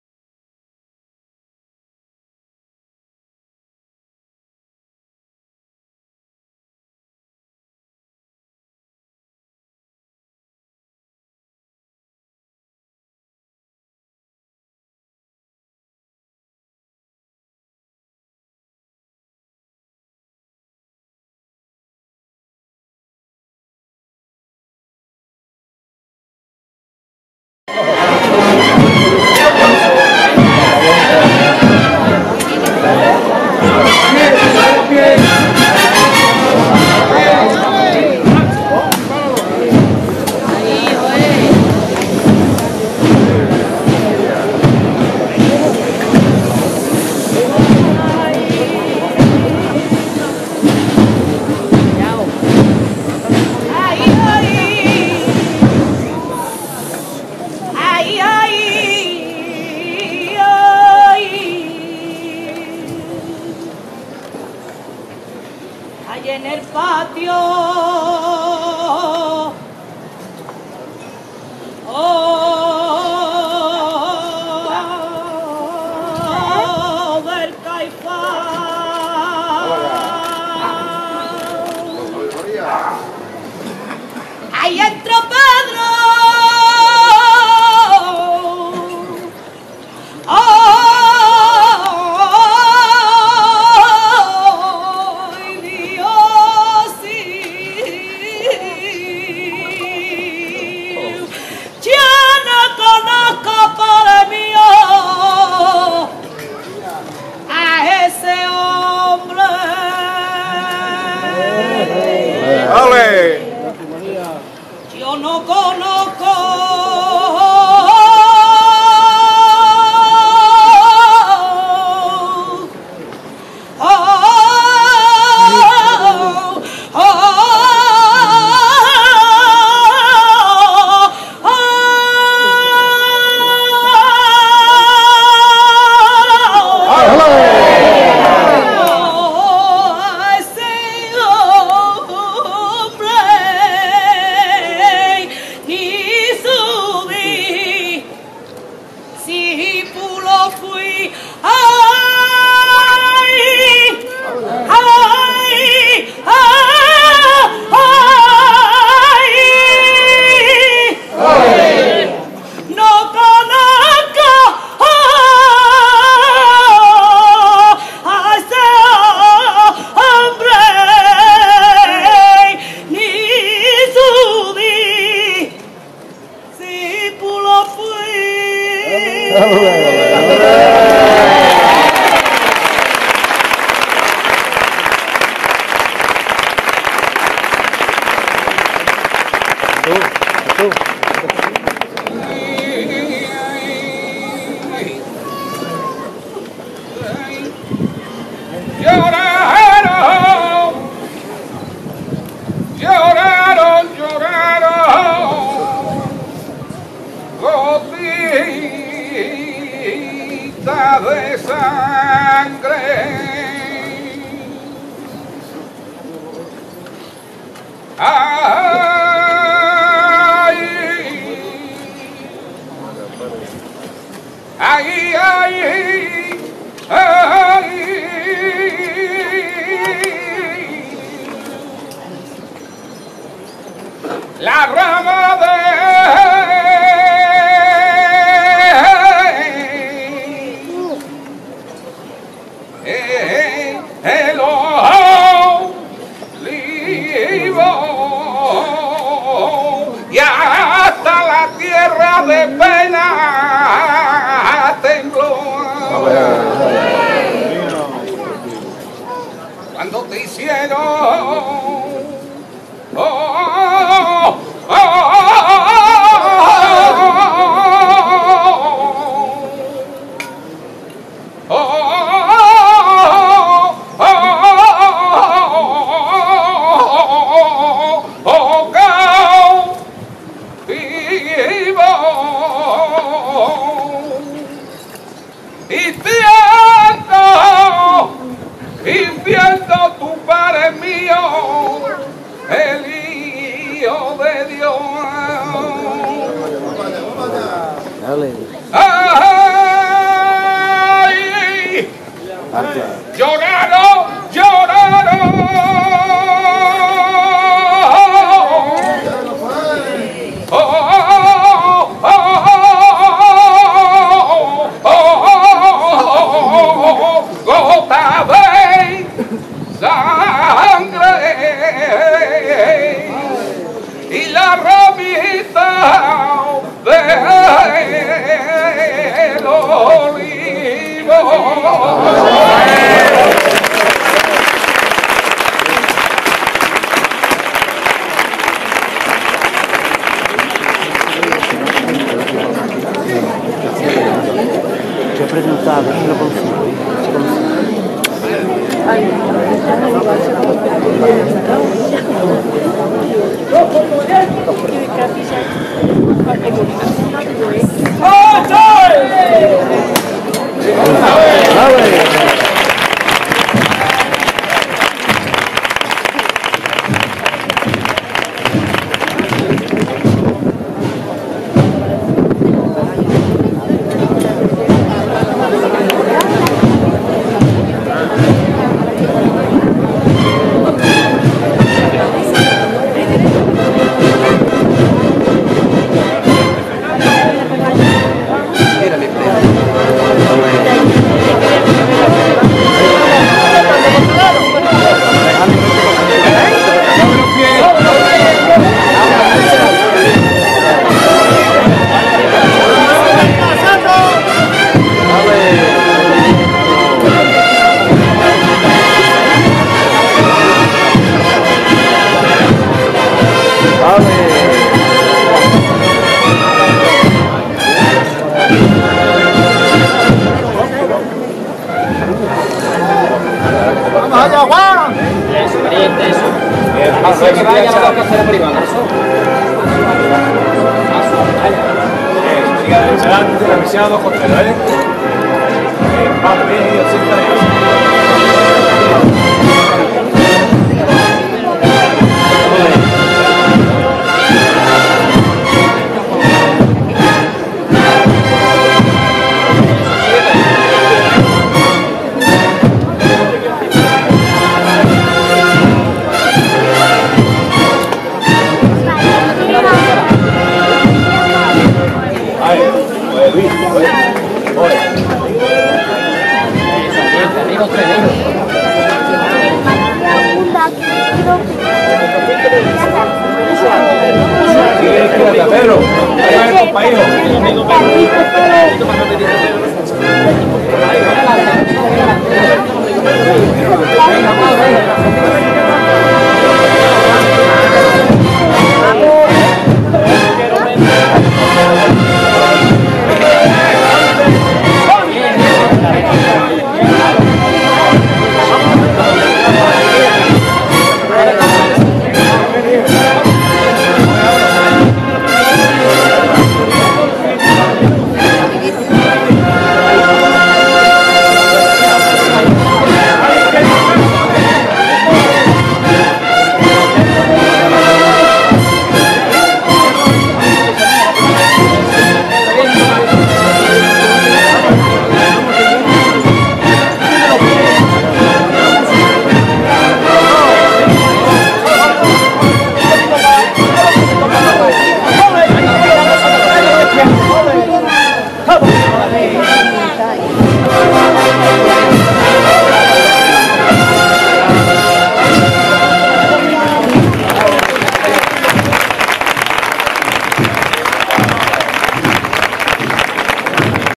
Jerez Saetas al Cristo del Prendimientio 17 de Abril 2019
Los pasos cerca de la calle Carpinteros con saetas muy bonitas